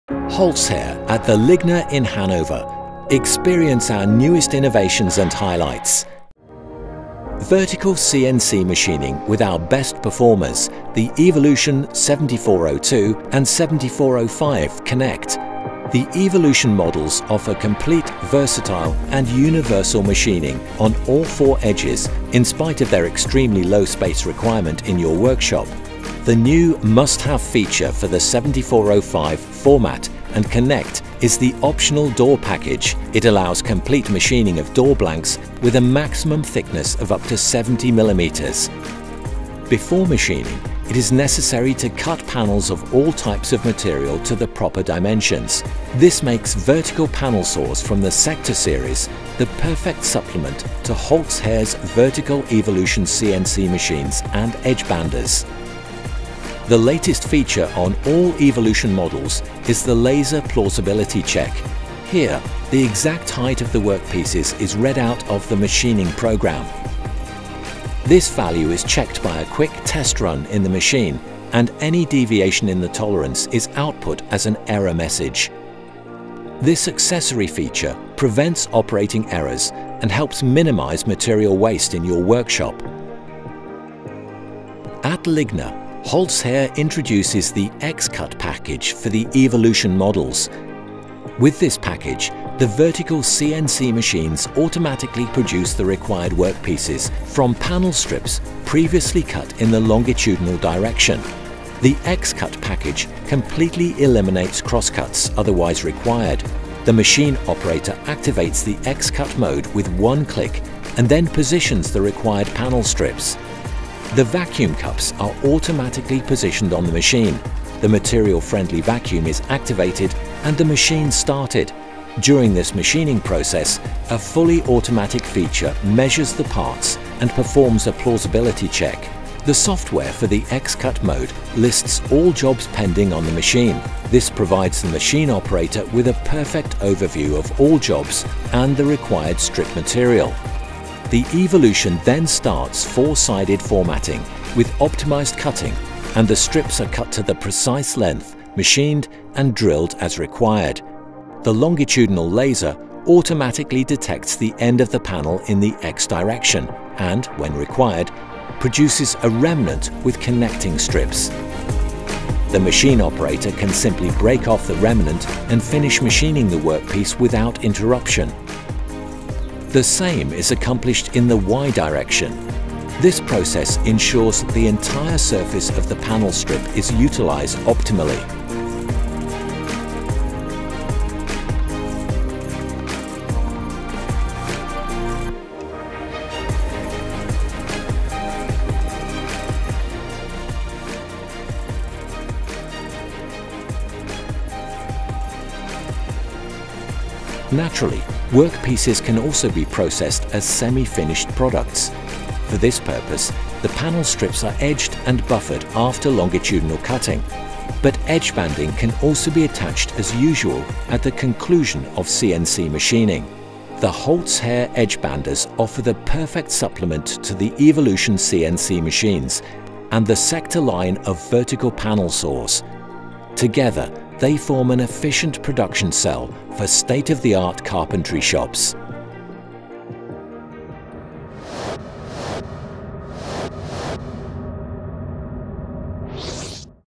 Cutting Panel Strips for Cabinetry and Furniture - Holz-Her Evolution
With xcut package the EVOLUTION automatically produces the required
workpieces from panel strips previously cut in the longitudinal direction.
The xcut mode completely eliminates cross-cuts otherwise required.
The xcut software lists all jobs pending on the machine. This provides
the machine operator with a perfect overview of all jobs and the required
strip material. The strips are cut to the precise length, formated on all
4 sides, and drilled as required. This process ensures that the entire
surface of the panel strips is utilized optimally.

In the so-called edge mode, previously edged strips can be formatted
tearfree to the required length.

This is how the optional xcut package makes the EVOLUTION 7402 and 7405
to the centre of your efficient carcase production.